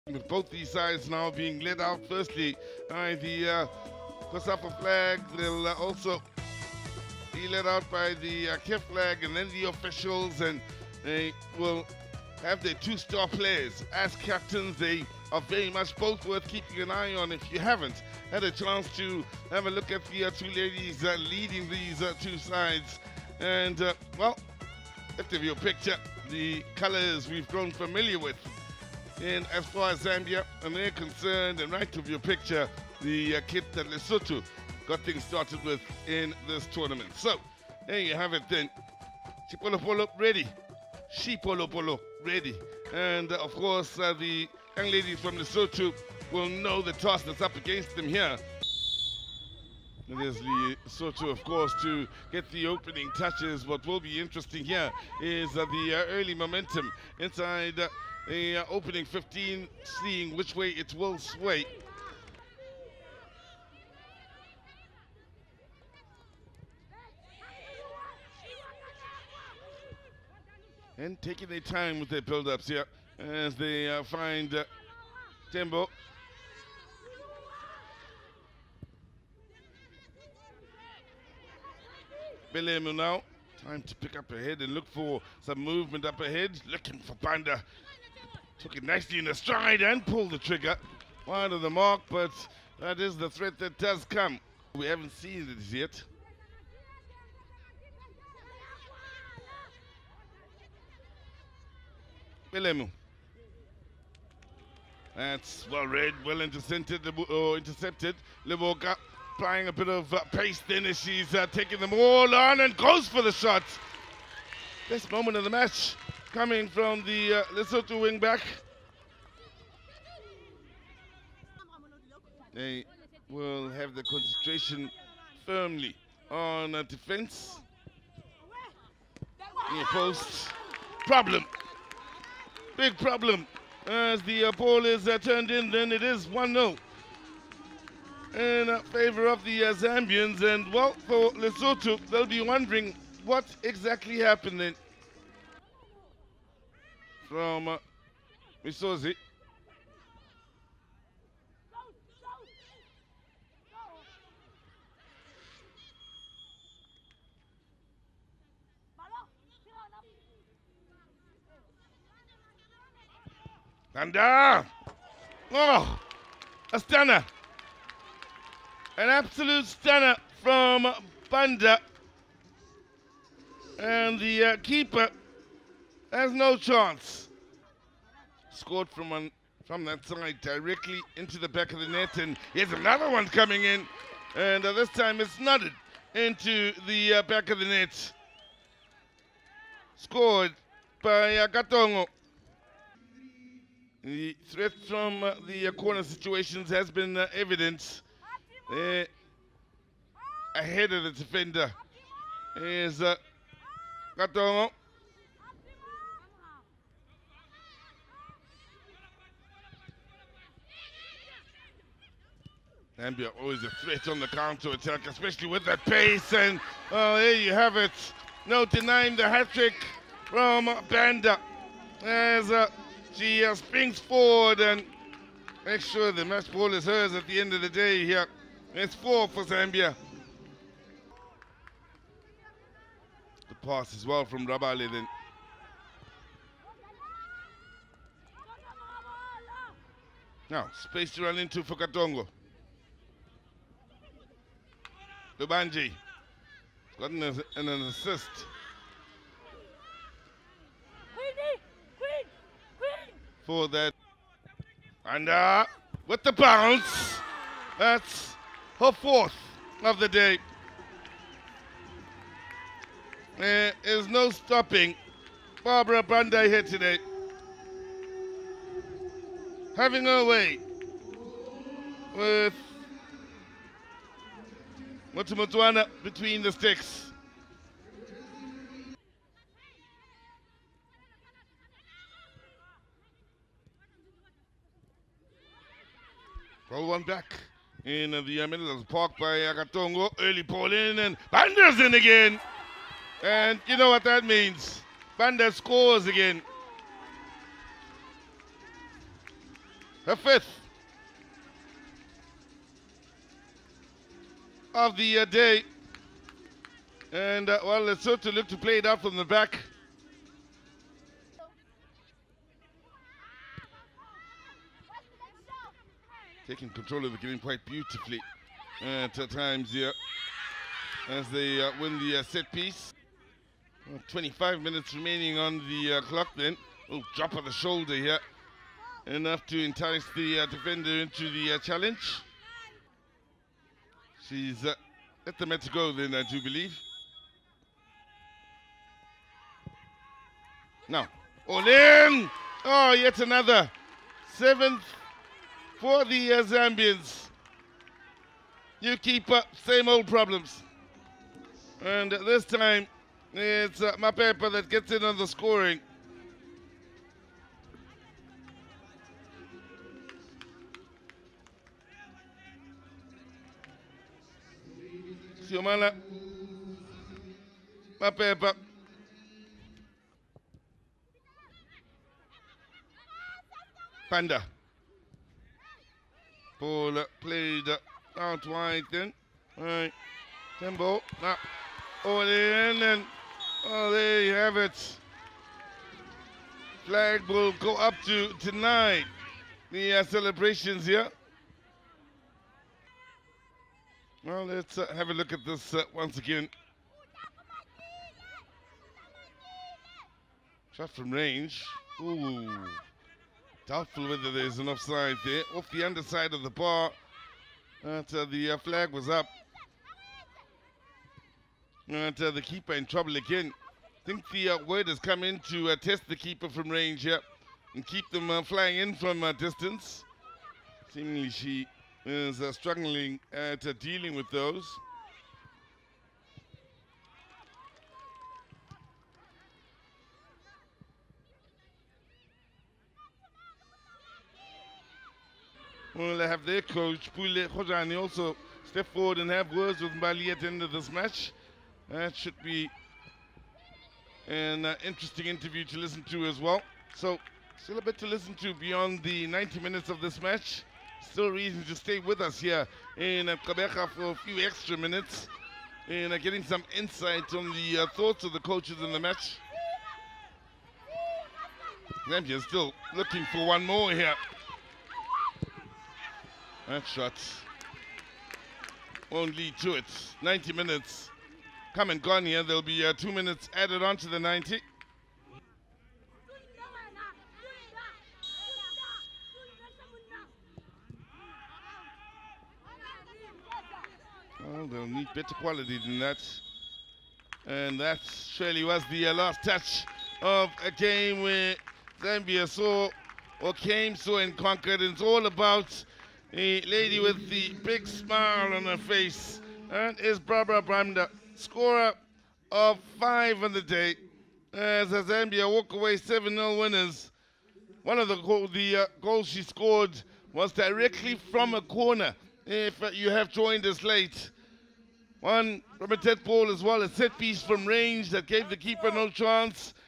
Copper Queens hit Lesotho for seven | Highlights | Zambia 7-0 Lesotho | COSAFA Women’s Championship
Take a close look at the 2022 Hollywoodbets COSAFA Women’s Championship Group B match as Zambia scored seven unanswered goals against Lesotho. Skipper Barbara Banda plundered five to walk away with a Player of the Match Award. A huge thank you to COSAFA for providing the FAZ Media Team with the match footage.